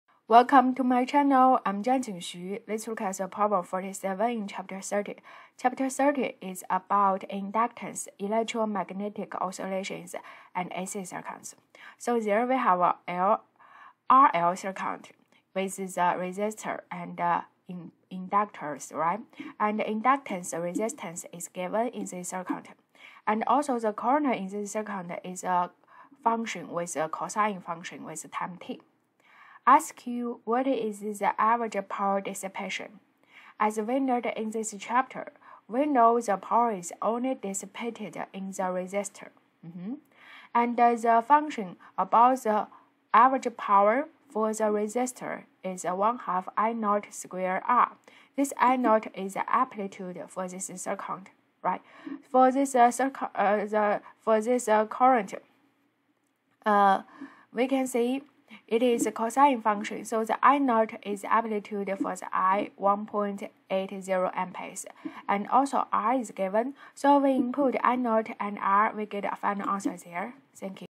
(30-47) A current I=1.80cos377t (I in amps, t in seconds, and the “ angle” is in radians) flows in a
(30-47) A current I=1.80cos377t (I in amps, t in seconds, and the “ angle” is in radians) flows in a series LR circuit in which L=3.85mH and R=1.35kΩ. What is the average power dissipation?